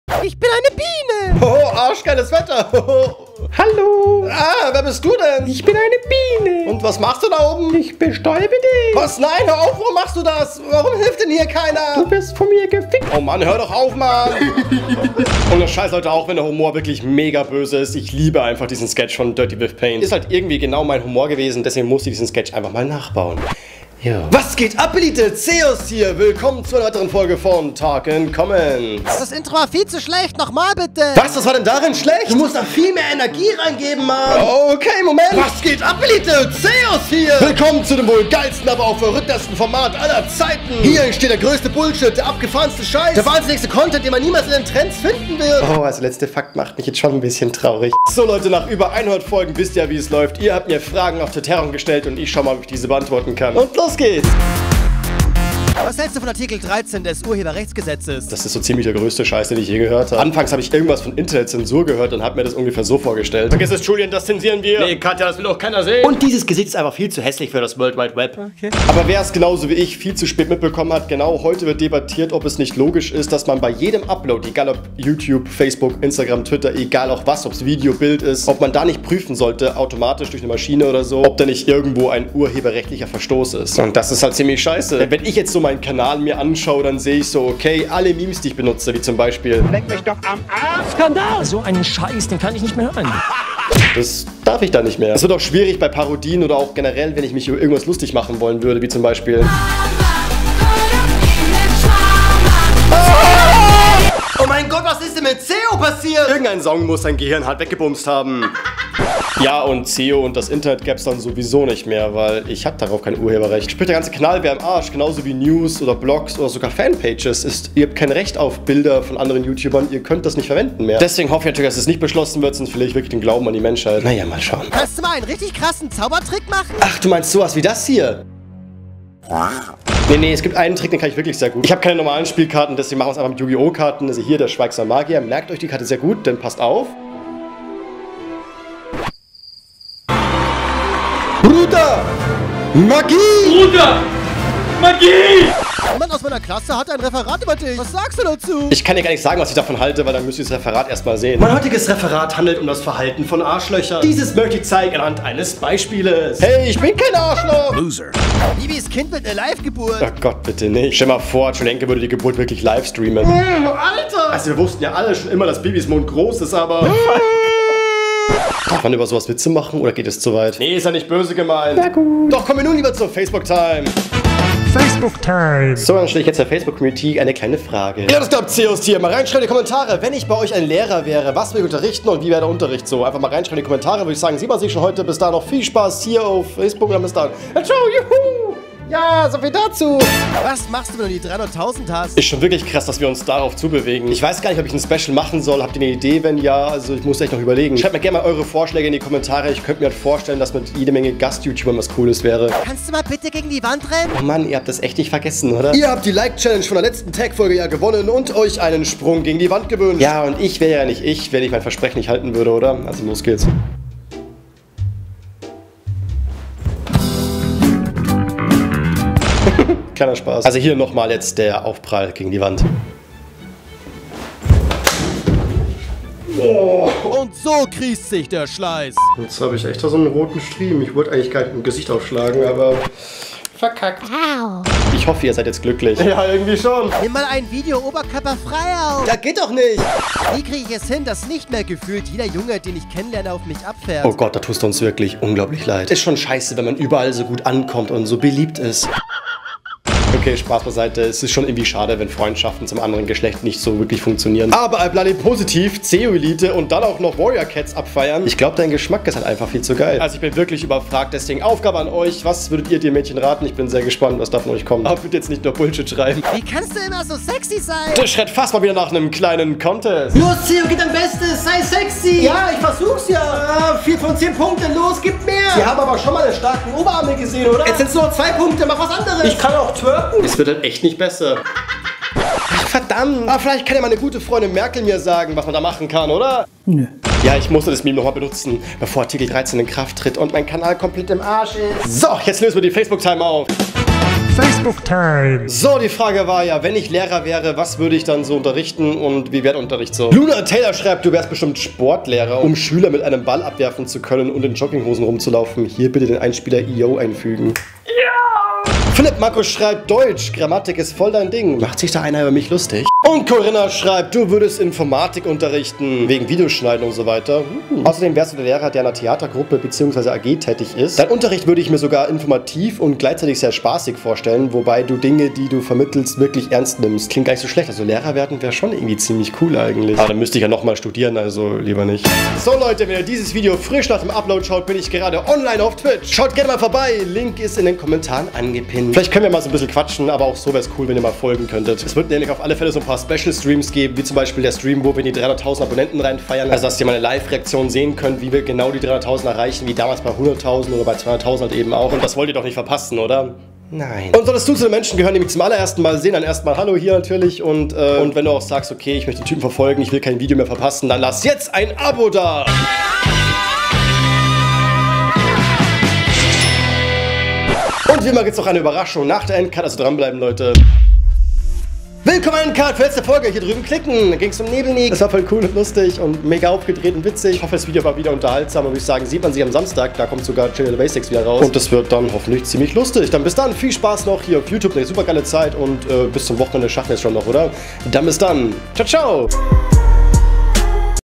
Zensiert das ALLES! - Zeo
Eure Fragen meine Antworten! :D
Unter anderem geht es auch um Artikel 13 und die damit verbundene Zensur über Youtube!

Hauptkanal:
GamingKanalVideo:
2. Kanal:

85615 Aßling